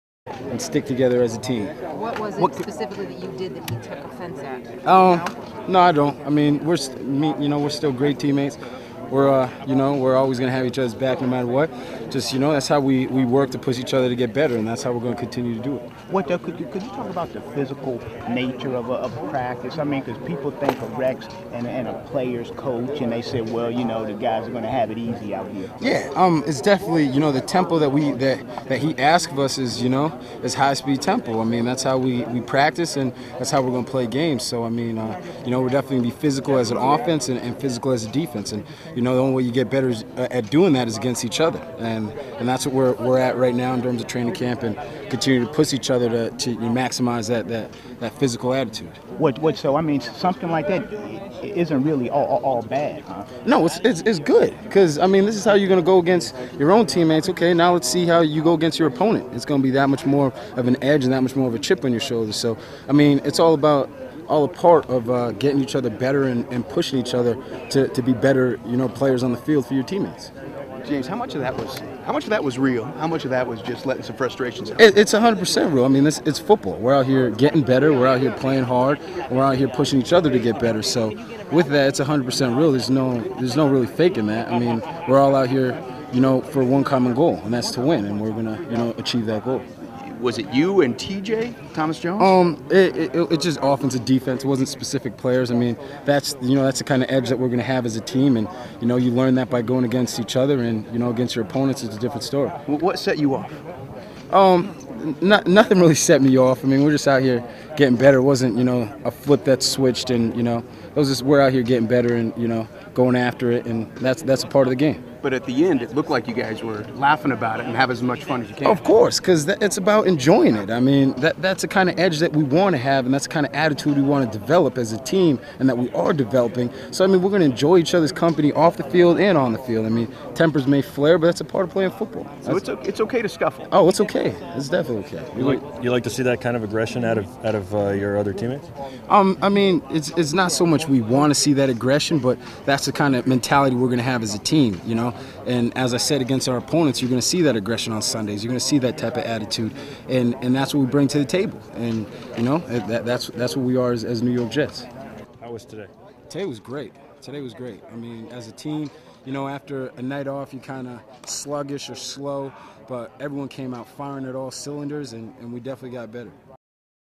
James Ihedigbo talks about brawl at Jets Camp
James talks about why this is a positive thing.